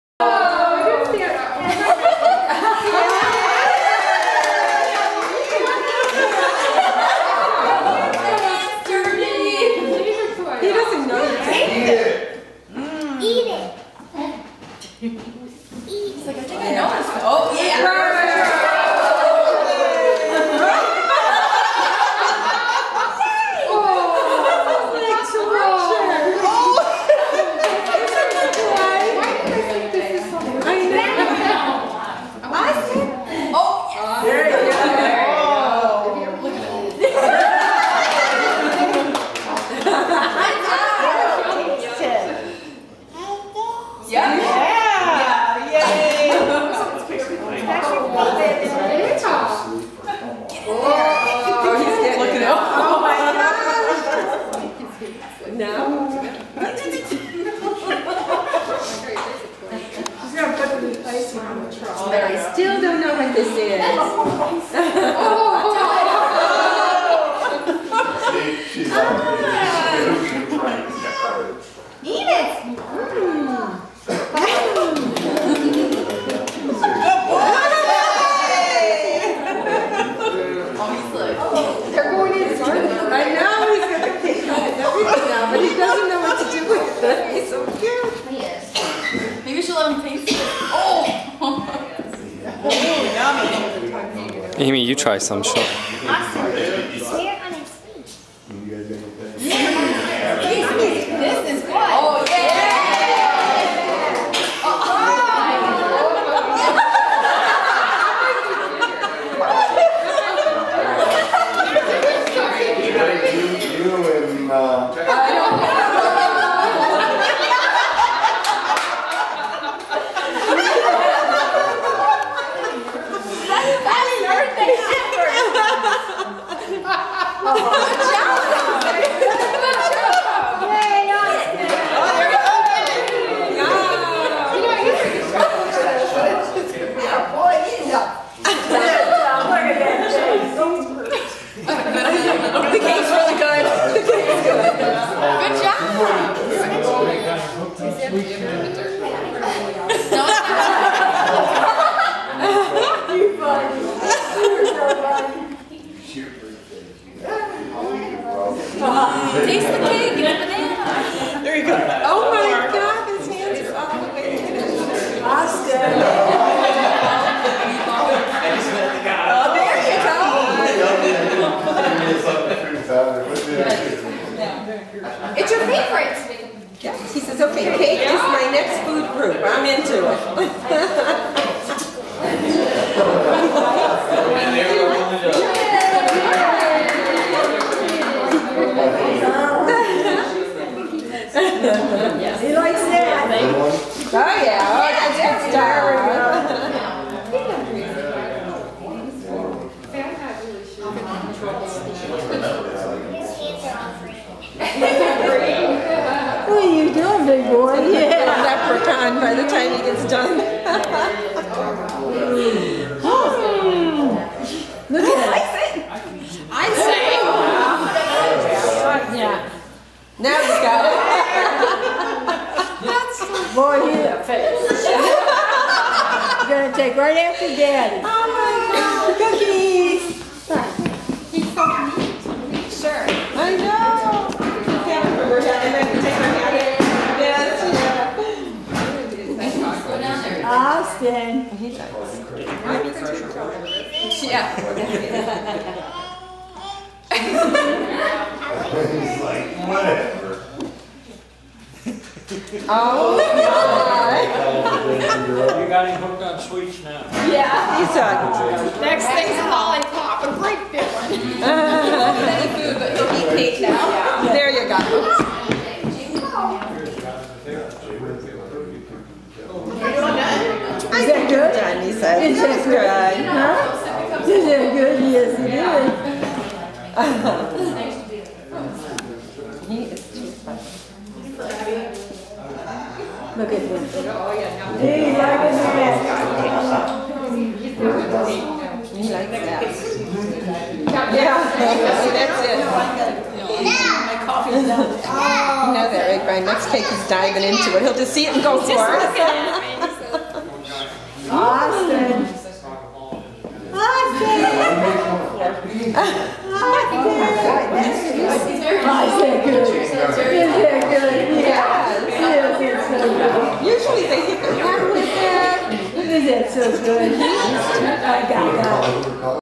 Austin Smashing his Cake at His 1st Birthday (1 year)
Austin eats and plays with his cake at his first birthday.  These are the first 5 minutes.  We let him play with it for 15-20 minutes total.  There was nothing left by the time he was done.